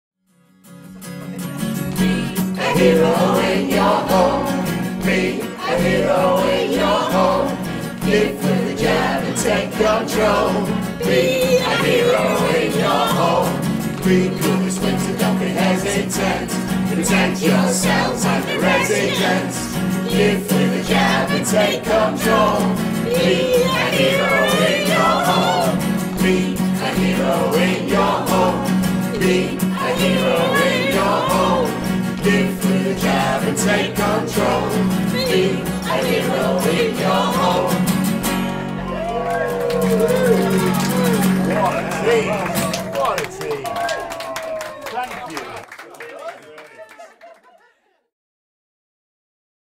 Four Seasons Flu Jab Jingle
A short, flu-jab-jingle video with the cheerful staff & residents of Four Seasons Health Care in Bolton.